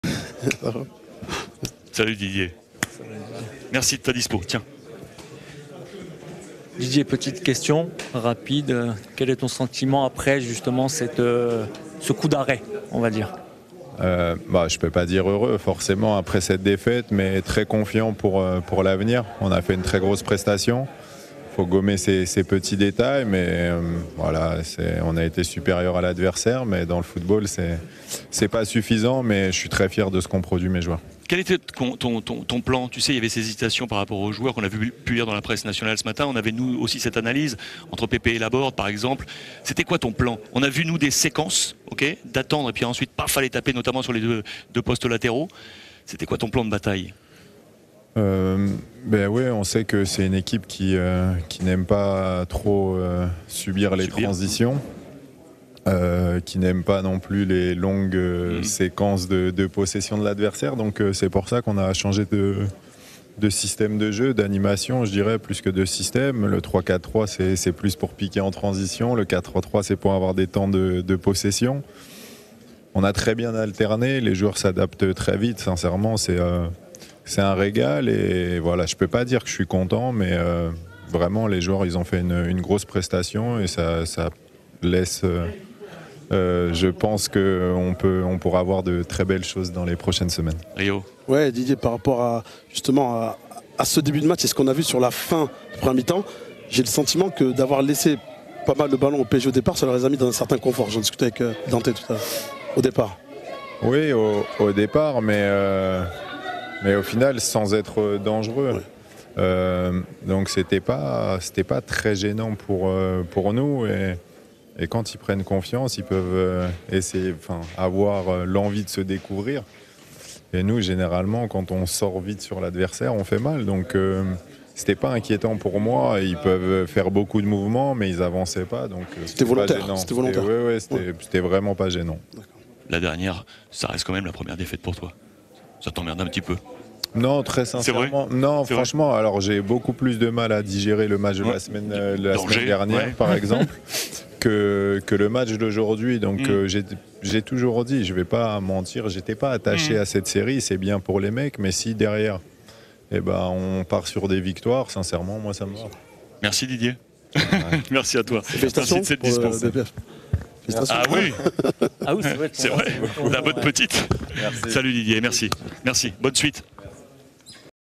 Nice/PSG - D. Digard : "Très confiant pour l'avenir" - BORD-TERRAIN
Malgré la défaite des joueurs niçois face au PSG, l'entraîneur Didier Digard s'est montré élogieux envers son équipe, dont le fond de jeu l'a nettement rassuré, après le match nul concédé à Angers.

Pour voir toutes nos interviews bord-terrain avant leur diffusion sur YouTube, téléchargez gratuitement l'application Free Ligue 1 Uber Eats :